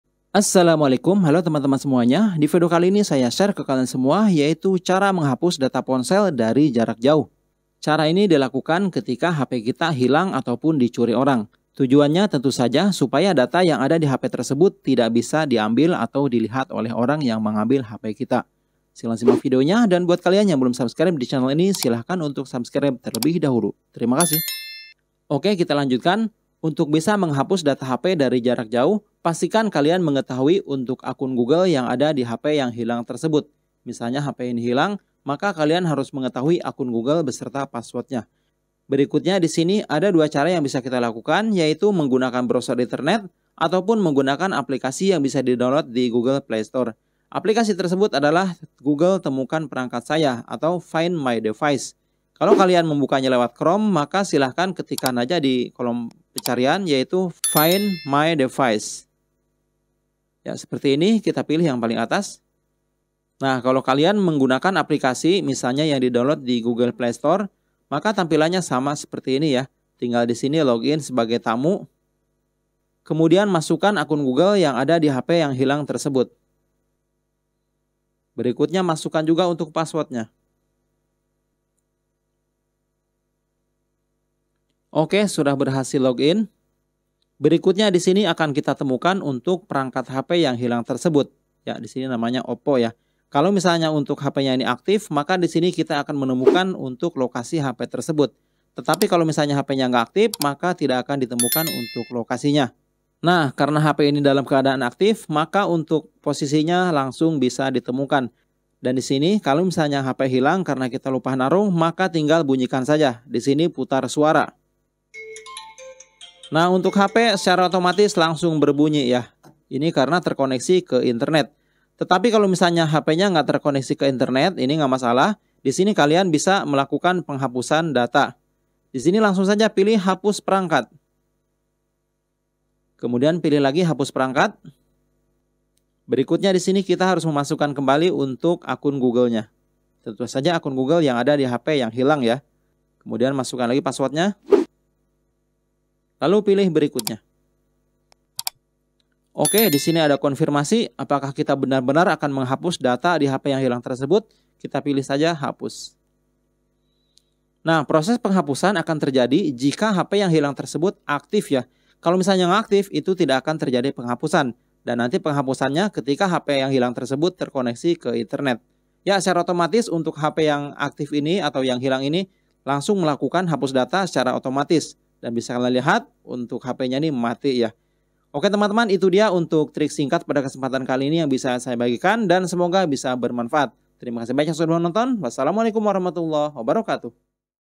Cara Menghapus Semua Data Di Hp Dari Jarak Jauh
Hai semuanya,di video kali ini saya share ke kalian semua yaitu cara menghapus semua data di ponsel dari jarak jauh.

Cara ini dilakukan ketika HP kita hilang ataupun dicuri orang,tujuannya tentu saja supaya data yang ada di HP tersebut tidak bisa diambil atau dilihat oleh orang yang mengambil hp kita .

Nah ada dua cara untuk melakukan, yaitu menggunakan aplikasi find device manager

Terimakasih